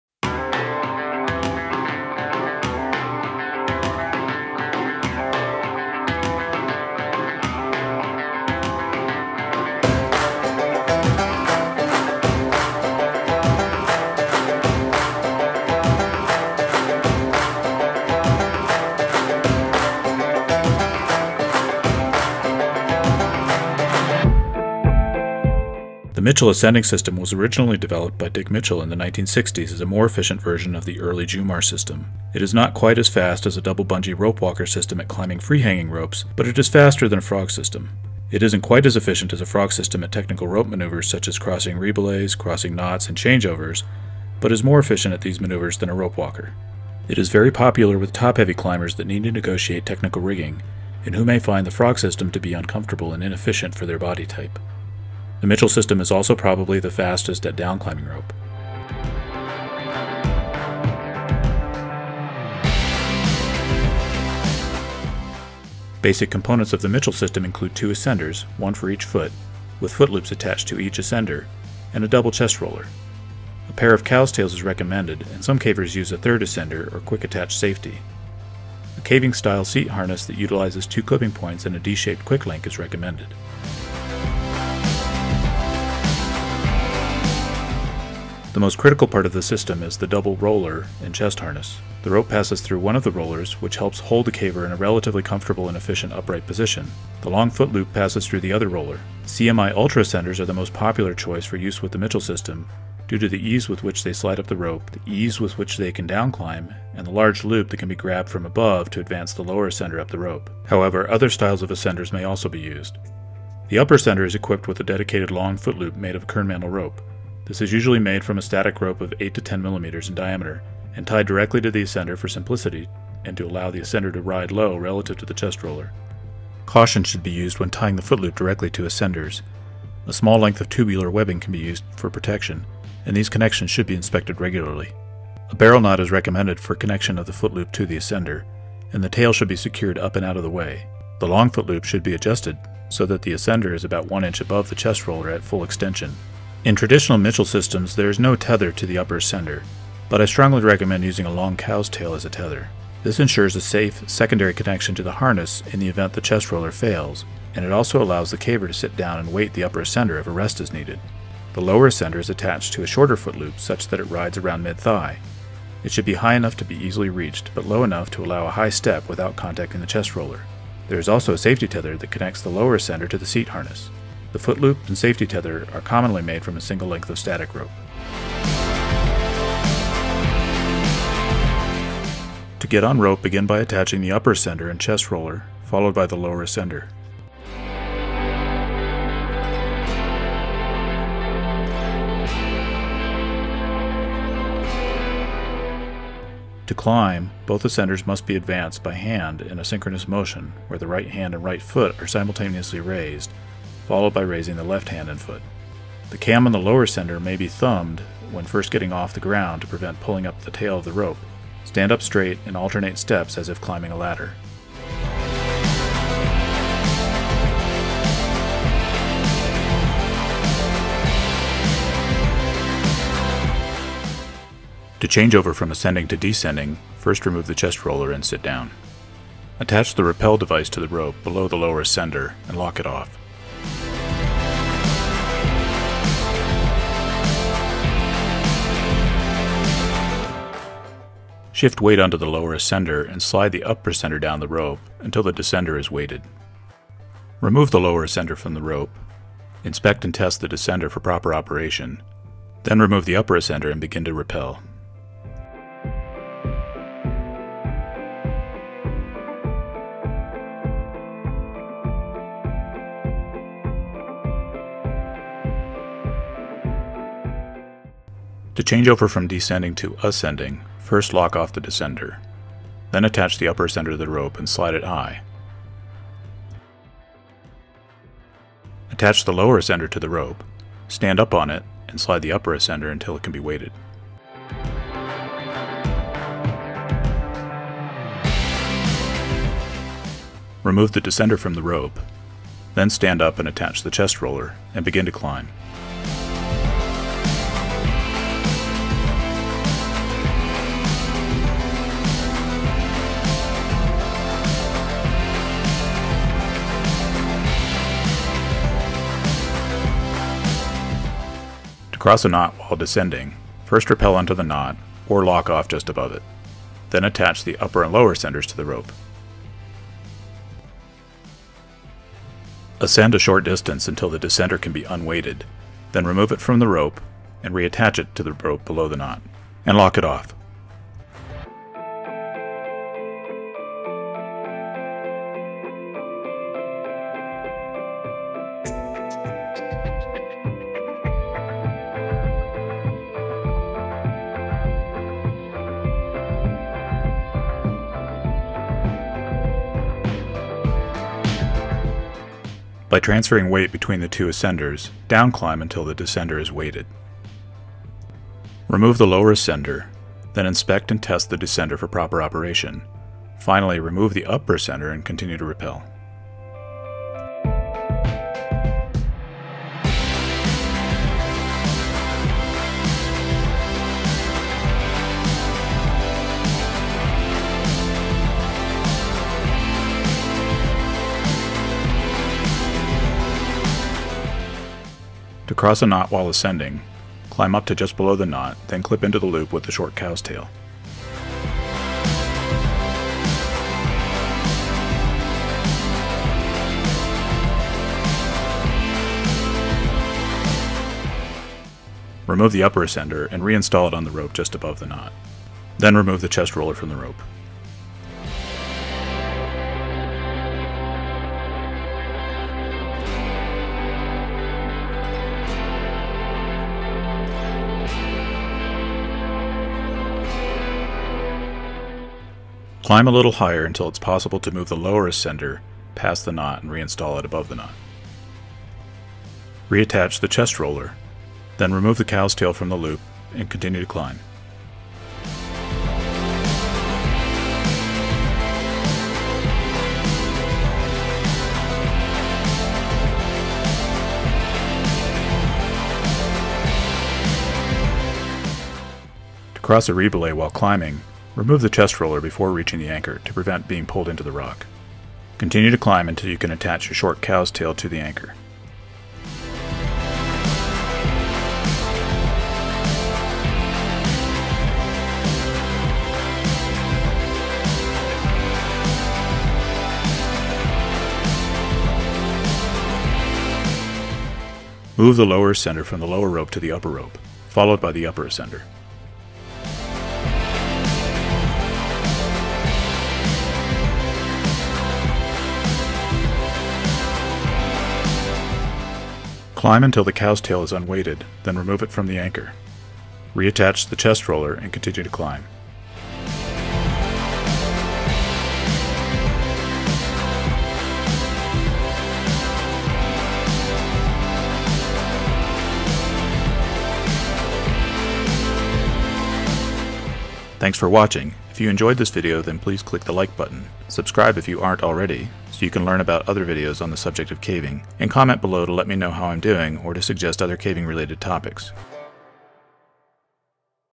Mitchell Ascending System for Caving
Caving Vlog #45
This video is an overview of the Mitchell system used to ascend ropes in single rope technique. Topics include equipment, climbing technique, change-overs, crossing rebelays, and crossing knots.